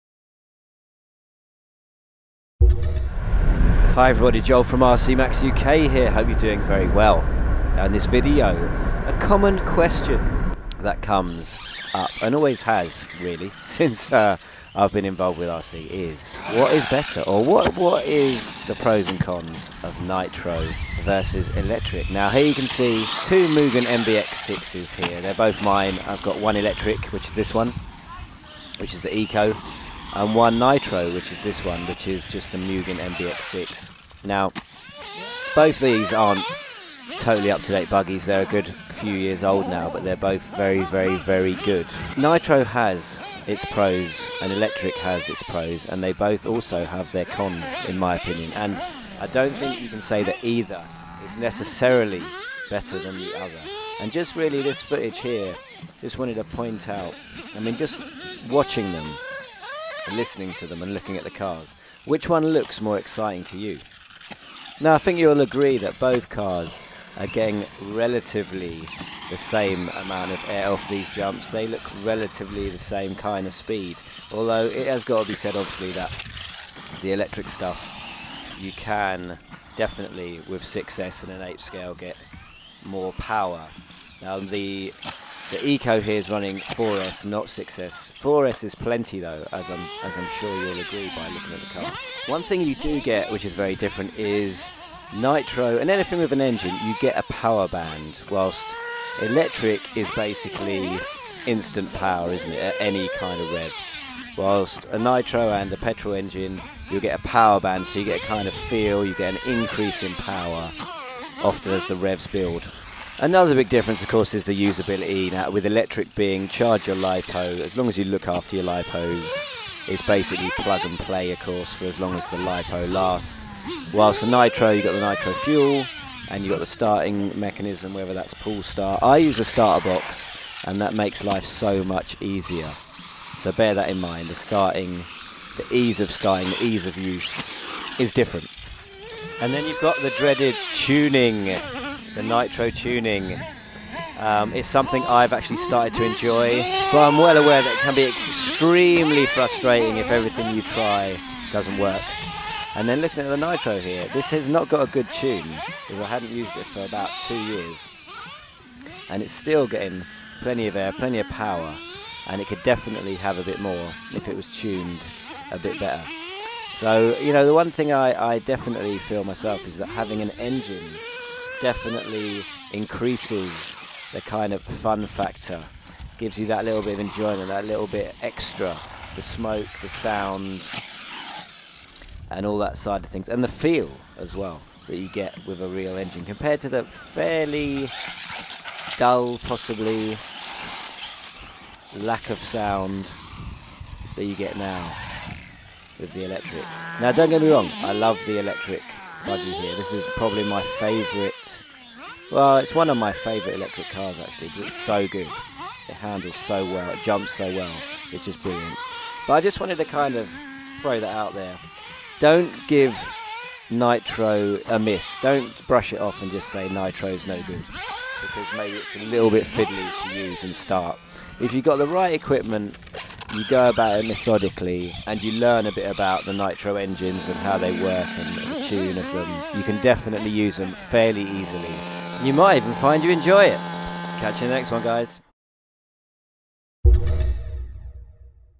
DON'T forget NITRO!!
Check out these 2 rc buggies... 1 nitro, 1 brushless... the same Mugen mbx6 platform... but a very different experience to use and drive.

Nitro and E have their pros and cons... neither are perfect and both give a different rc experience that the other just can't. Get both and a petrol car too! Don't close off so much Rc enjoyment by just running 1 form of propulsion and moaning at others for loving something you don't!

Give it a go and ENJOY RC!